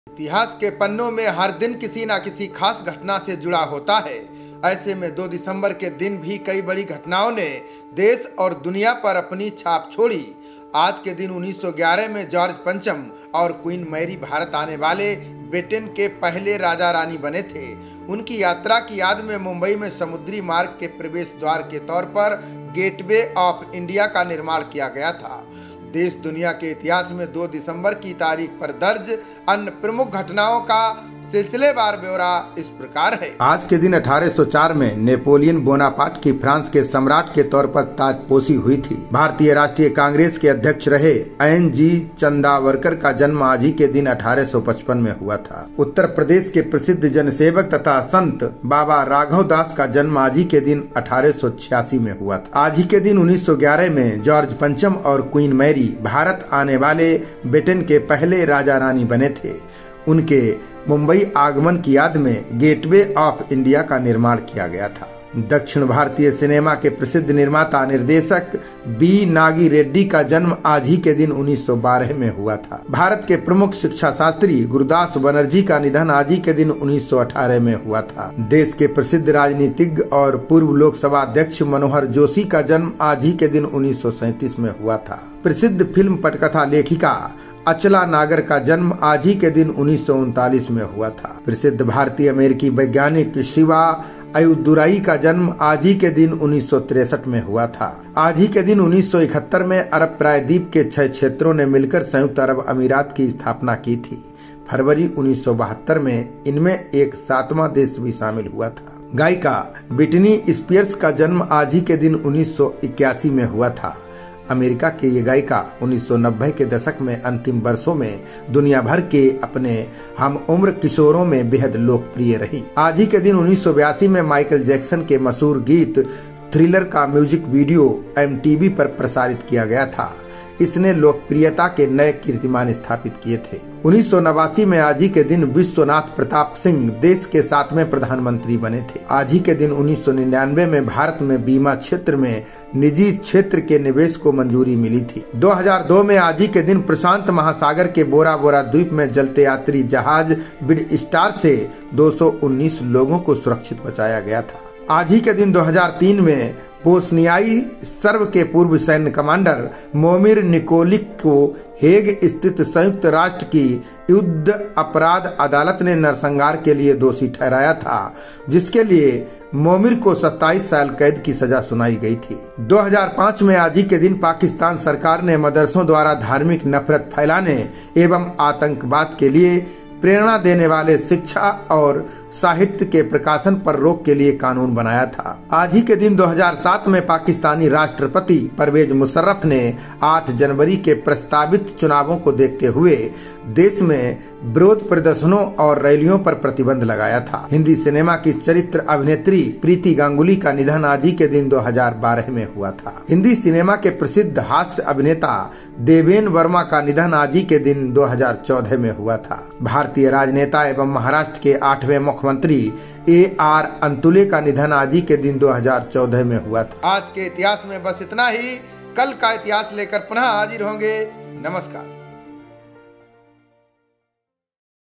02-12-2022 | 02 दिसंबर को घटने वाली बड़ी घटनाएं | Today History | Event | Janpravad...
आज के दिन 1804 में नेपोलियन बोनापार्ट की फ्रांस के सम्राट के तौर पर ताजपोशी हुई थी।
2
भारतीय राष्ट्रीय कांग्रेस के अध्यक्ष रहे एन. जी. चन्दावरकर का जन्म आज ही के दिन 1855 में हुआ था।
3
उत्तर प्रदेश के प्रसिद्ध जनसेवक तथा संत बाबा राघवदास का जन्म आज ही के दिन 1886 में हुआ था।
4
आज ही के दिन 1911 में जॉर्ज पंचम और क्वीन मैरी भारत आने वाले ब्रिटेन के पहले राजा-रानी बने थे। उनके मुंबई आगमन की याद में गेटवे आॅफ इंडिया  का निर्माण किया गया था।
5
दक्षिण भारतीय सिनेमा के प्रसिद्ध निर्माता-निर्देशक बी. नागी रेड्डी का जन्म  आज ही के दिन 1912 में हुआ था।
6
भारत के प्रमुख शिक्षाशास्त्री गुरुदास बनर्जी का निधन आज ही के दिन 1918 में हुआ था।
7
देश के प्रसिद्ध राजनीतिज्ञ और पूर्व लोकसभा अध्यक्ष मनोहर जोशी का जन्म  आज ही के दिन 1937 में हुआ था।
8
प्रसिद्ध फिल्म पटकथा लेखिका अचला नागर का जन्म आज ही के दिन 1939 में हुआ था।
9
प्रसिद्ध भारतीय-अमेरिकी वैज्ञानिक शिवा अय्यदुरई का जन्म आज ही के दिन 1963 में हुआ था।
10
आज ही के दिन 1971 में अरब प्रायद्वीप के छह क्षेत्रों ने मिलकर संयुक्त अरब अमीरात की स्थापना की थी। फरवरी 1972 में इनमें एक सातवां देश भी शामिल हुआ था।

Janpravad Magazine
Janpravad Tourism
Janpravad Superfast